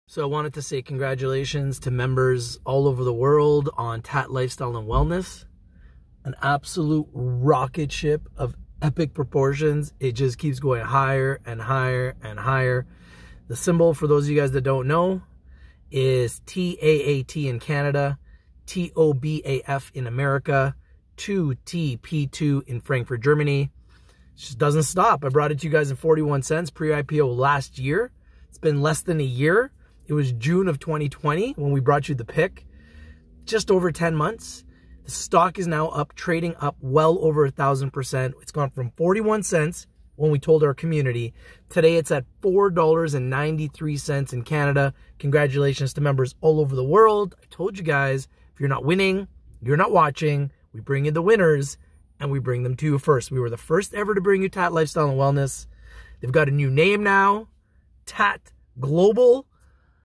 Taat Global Alternatives Inc. (TAAT, Tobaf, 2TP2)
Taat Global Initiatives - April 28, 2021 - RICH PICKS DAILY - TAAT ™ GLOBAL ALTERNATIVES INC. (CSE: TAAT) (OTCQX: TOBAF) (FRANKFURT: 2TP2) (the “Company” or “TAAT ™ ”) announces that as of 9:30 am EDT today (Wednesday, April 28, 2021), the Company’s name has officially changed from TAAT™ Lifestyle & Wellness Ltd. (the “Former Name”) to TAAT™ Global Alternatives Inc. (the “Updated Name”), as announced in a press release dated Monday, April 26, 2021 .
No change has been made to the Company’s ticker symbol as a result of this name change, and the Company’s common shares will continue to trade under the ticker “TAAT” on the Canadian Securities Exchange. The CUSIP number assigned to the Company’s shares following the name change to the Updated Name is 87320M101 (ISIN: CA87320M1014).

No action is required to be taken by shareholders with respect to today’s name change. Outstanding share and warrant certificates are not affected by the name change and do not need to be exchanged.